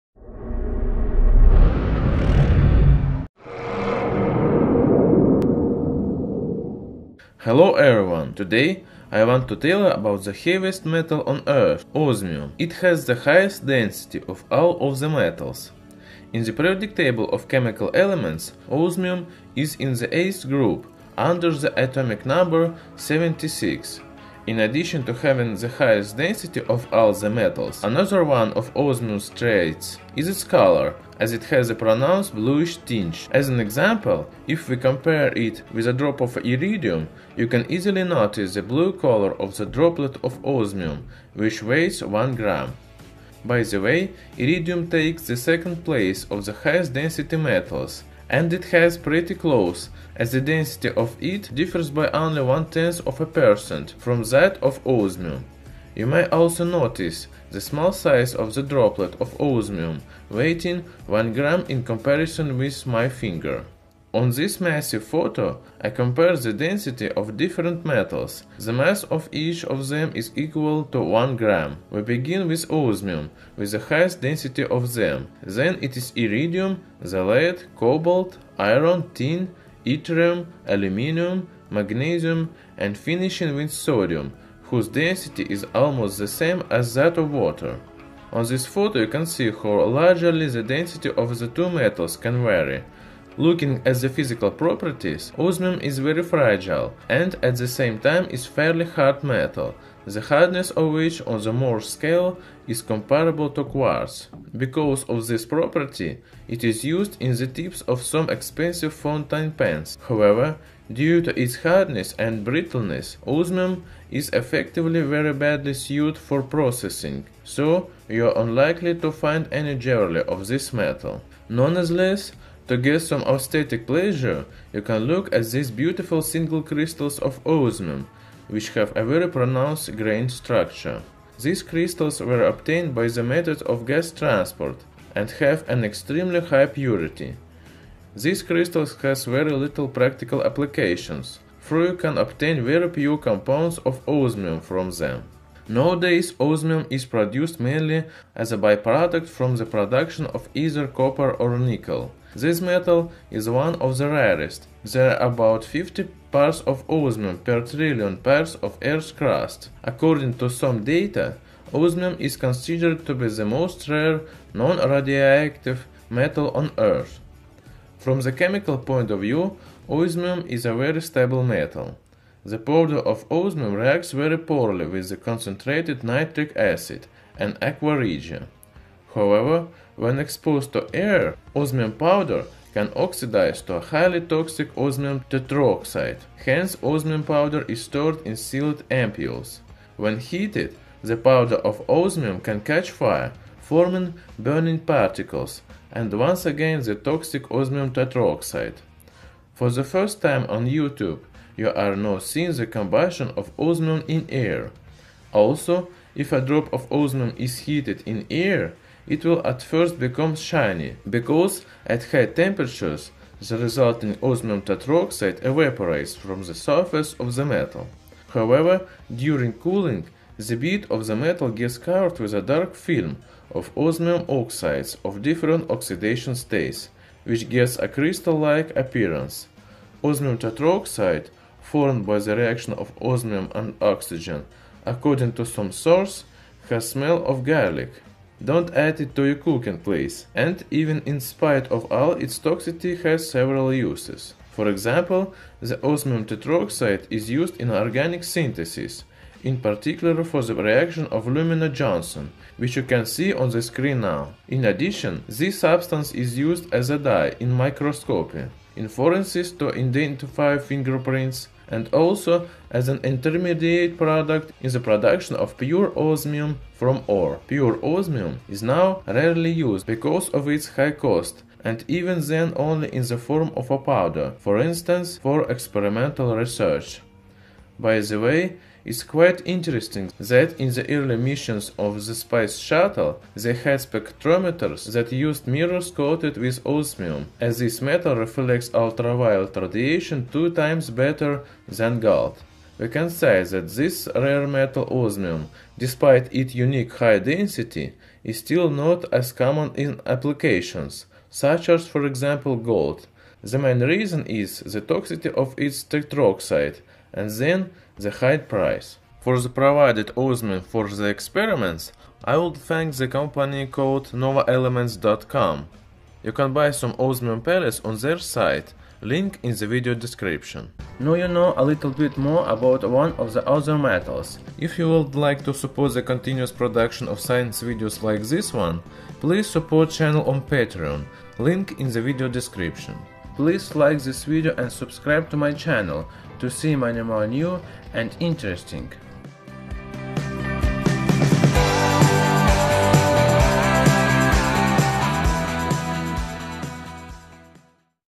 Osimi - Kim loại nặng nhất trên Trái Đất! - (Thoisoi VIETSUB)
Osimi là kim loại nặng nhất trong tất cả các kim loại từng được biết ( mật độ = 22.587 g/cm³), kim loại này khá cứng nhưng cũng khá giòn, nhiệt độ nóng chảy rất cao, khoảng 3030 °C. Đứng thứ năm trong bảng xếp hạng các nguyên tố có nhiệt độ nóng chảy cao nhất. Osimi được hai nhà hóa học người Anh “Smithson Tennant” và “William Hyde Wollaston” tìm ra năm 1804. Đặt biệt Osimi Tetrôxit có độc tính mạnh, nên hãy cẩn thận khi làm việc với hợp chất này nhé!

Subtitled and Edited by: Chemical Elements
“Video gốc thuộc về kênh Thoisoi 2”